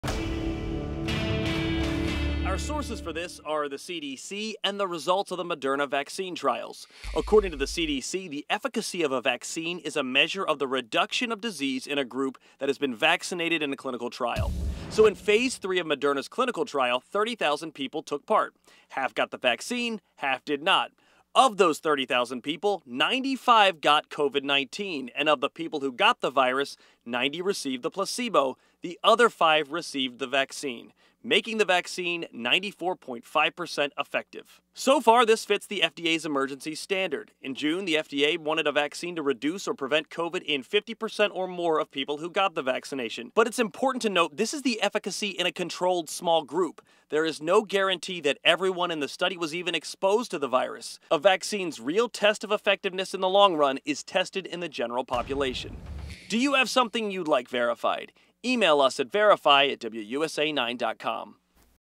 VERIFY: What does Moderna vaccine's 94.5% efficacy mean?
Moderna released the initial results of their Phase III clinical trial for a coronavirus vaccine. We breakdown what efficacy is.